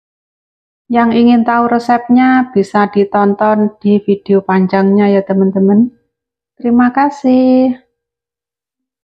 resep ikan sarden kalengan
assalamualaikum...
hallo semua jumpa lagi dengan channel bunda any, di video kali ini menu bunda any ikan sarden kalengan.yg ingin tau bagaimana cara memasaknya simak Vidio panjang nya terimakasih.